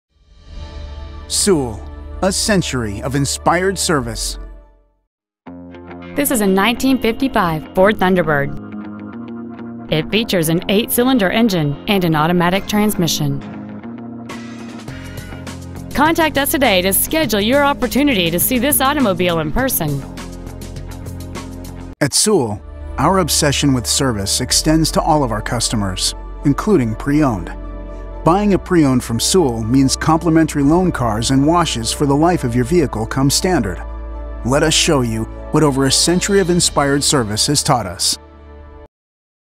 1955 Ford Thunderbird San Antonio TX 78230
We are proud to present this 1955 Ford Thunderbird .

 Year : 1955

 Make : Ford

 Model : Thunderbird

 Engine : 292 V8

 Trans . : Automatic

 Exterior : Black

 Miles : 73,474

 Interior : Black

 11001 IH-10 W

 Mission

 Values

 1955 Ford Thunderbird San Antonio TX